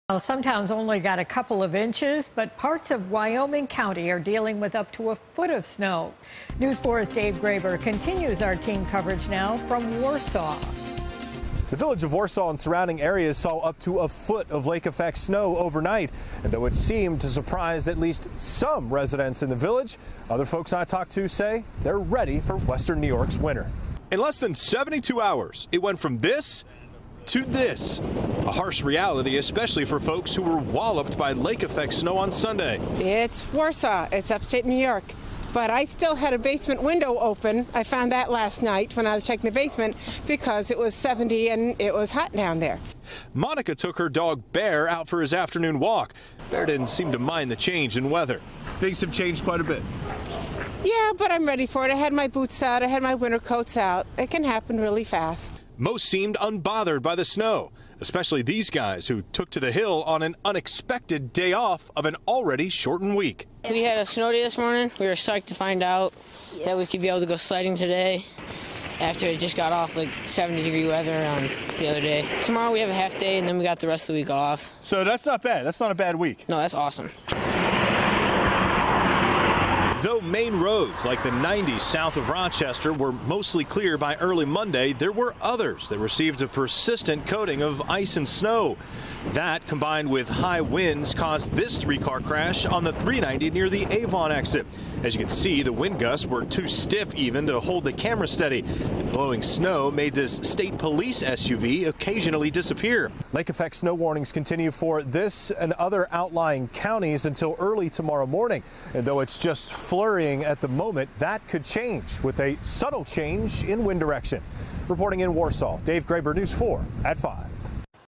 Up to a foot of snow in some parts of Wyoming County
School was called for kids in the village of Warsaw; snow keeps crews busy